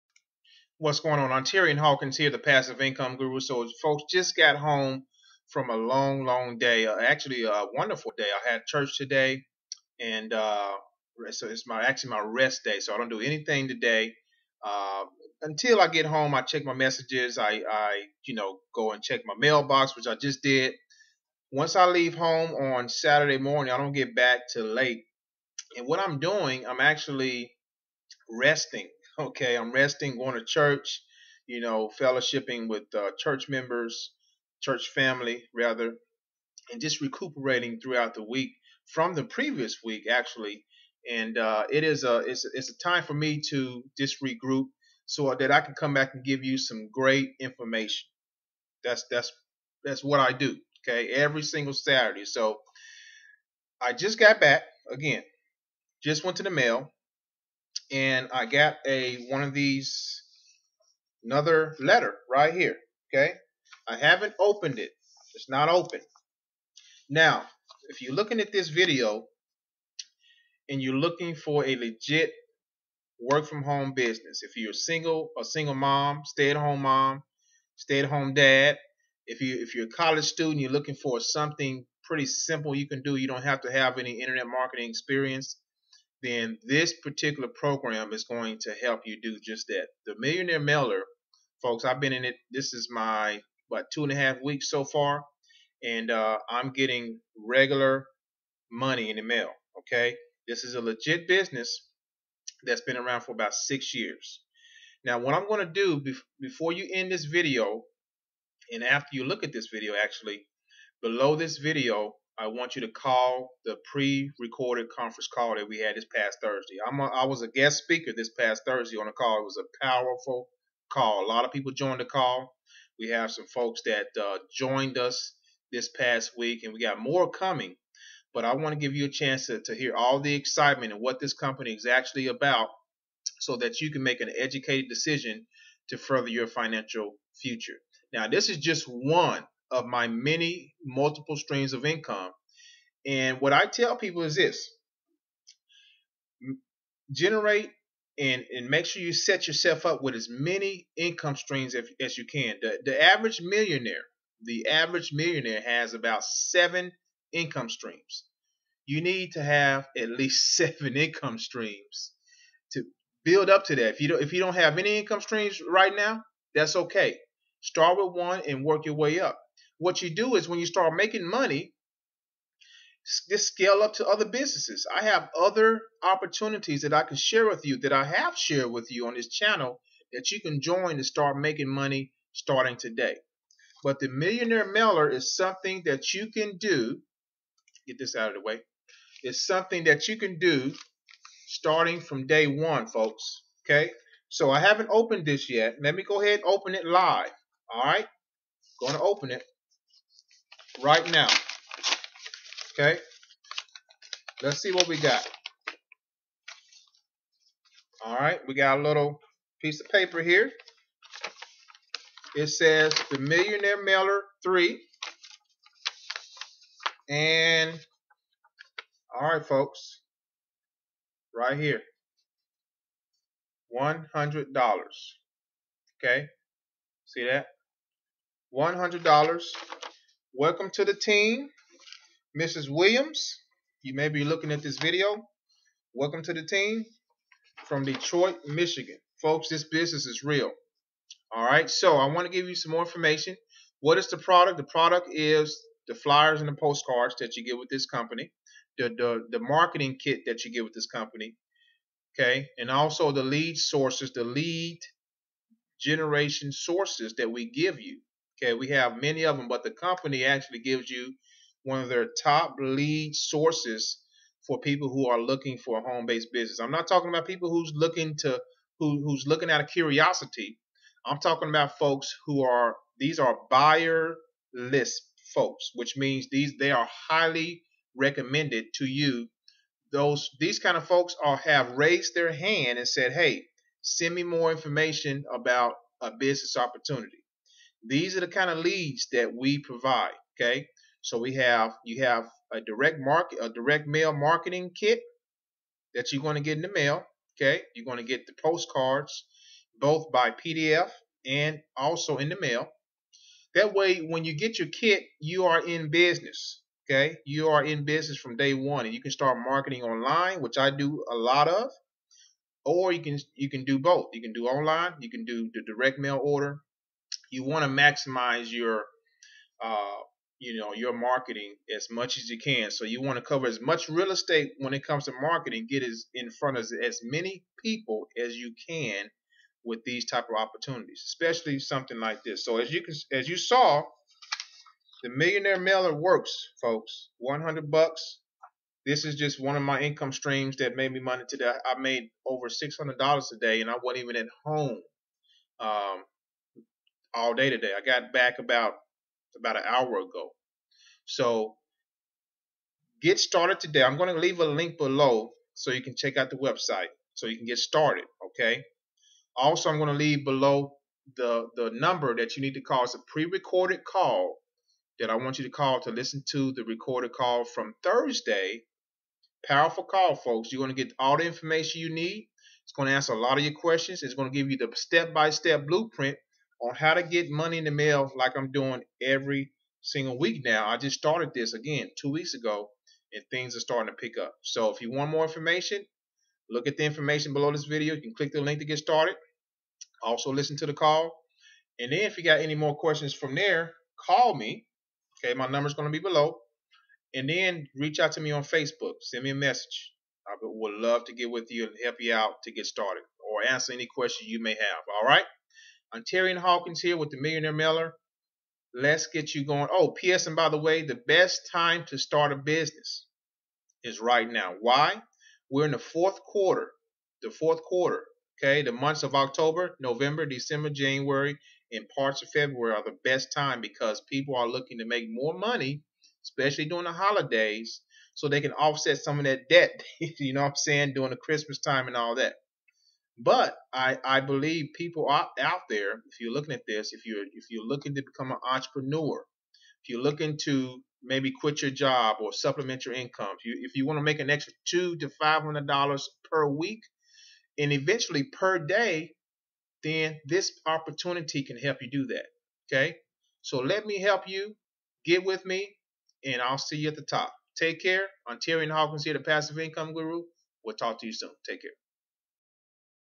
"Millionaire Mailer" Best Legit Work From Home Business Opportunity- Earn $100-$200/Day Online
If you are looking for a legit work from home business opportunity, then the millionaire mailer is the right one for you. Learn how to make extra income from home with the millionaire mailer.